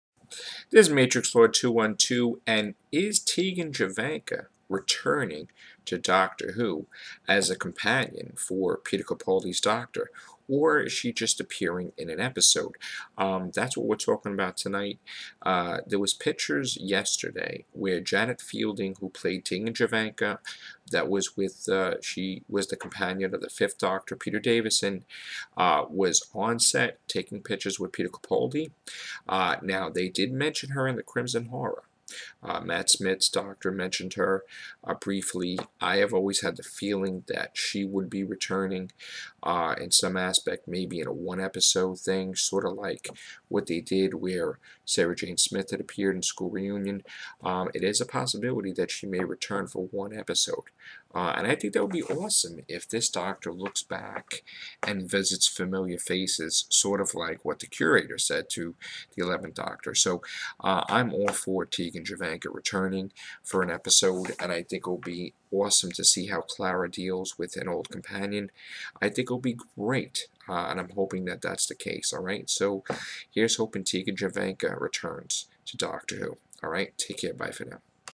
Doctor Who Series 8: Tegan Jovanka Returning?
Will the 5th Doctor's companion return?